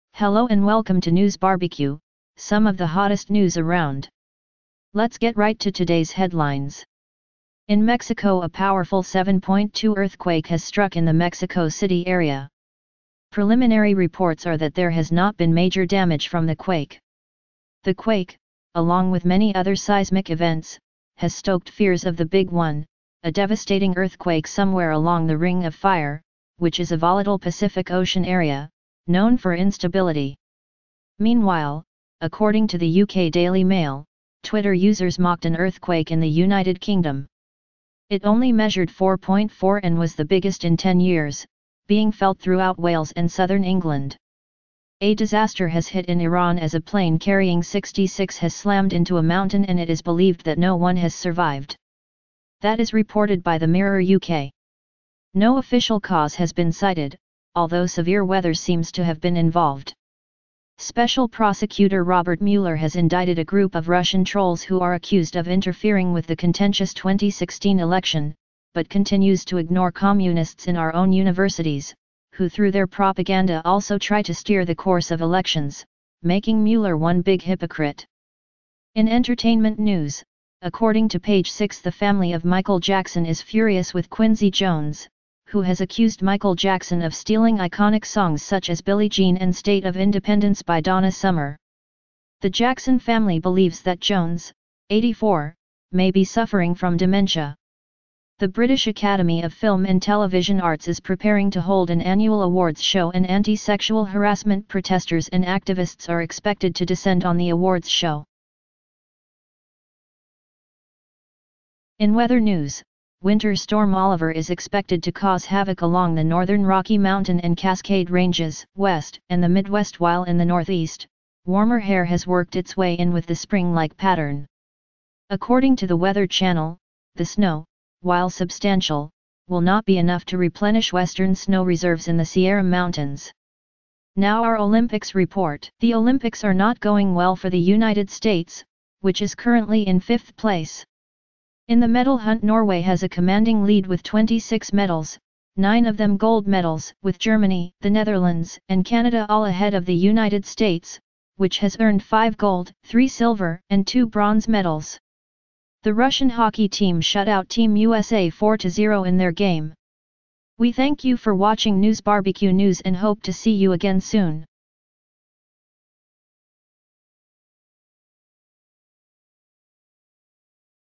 NewsBarbecue com news test broadcast
This is a test video for my new website, Newsbarbecue.com. I have will put it on the main page later. I made this video to test the most critical factor for me which is time. I have not finished the home page yet but I hope to have things ready in the next couple of days.  SJW's can get bent if they don't like it.